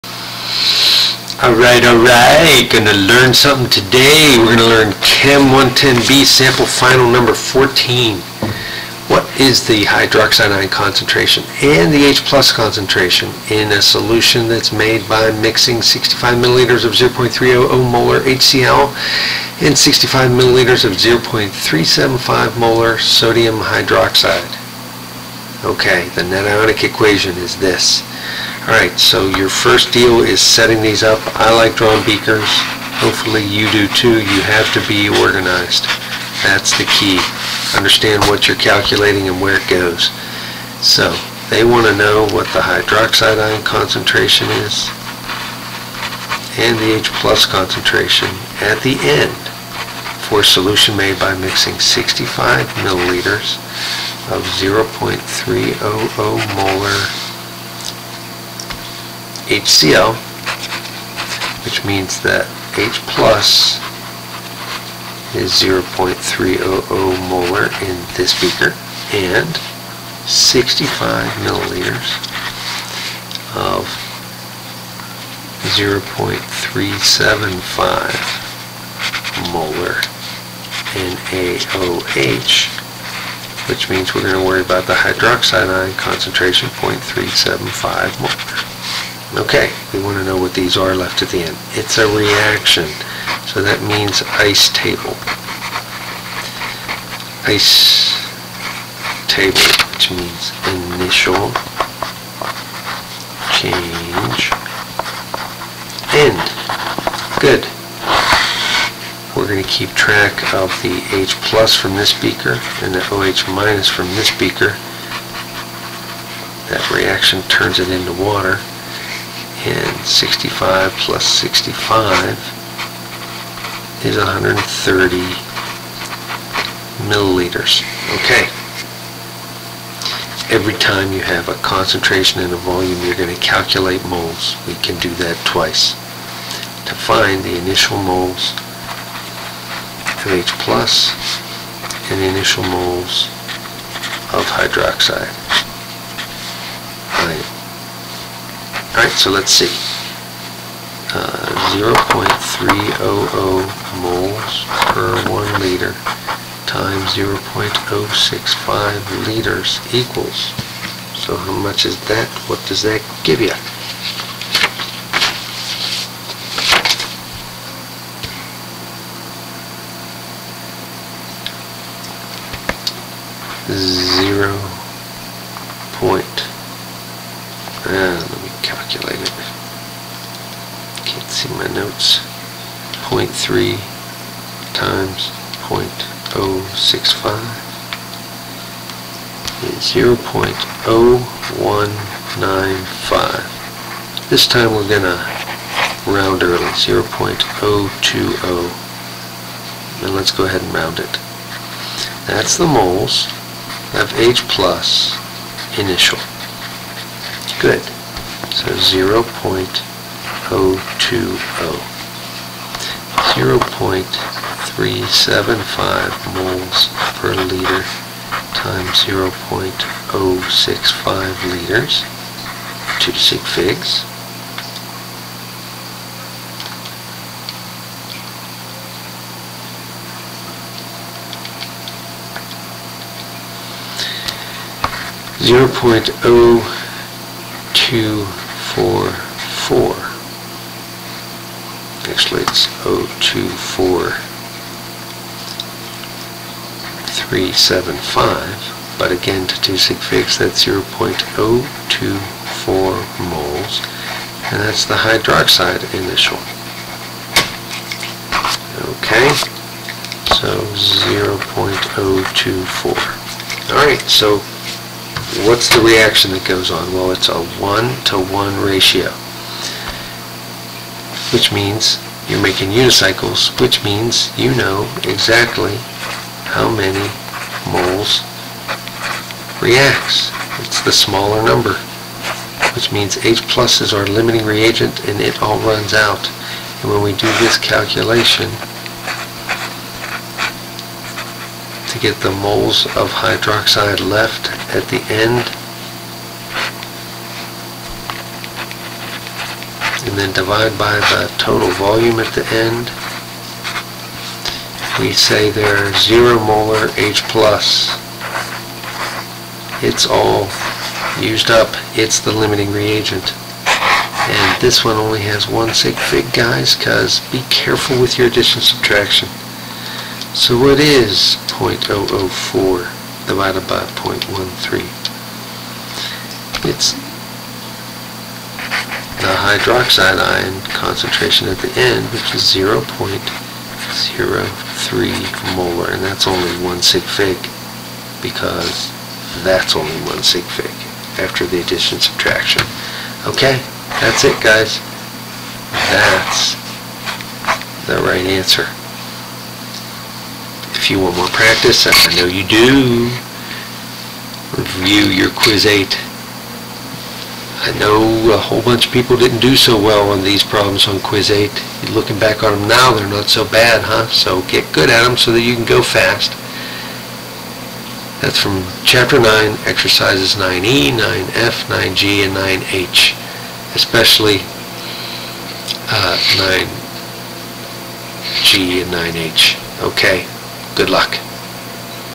Chem 110B Sample Final #14
Beaker problem! Concentrations of both reactants in the final solution is determined using ICE TABLE!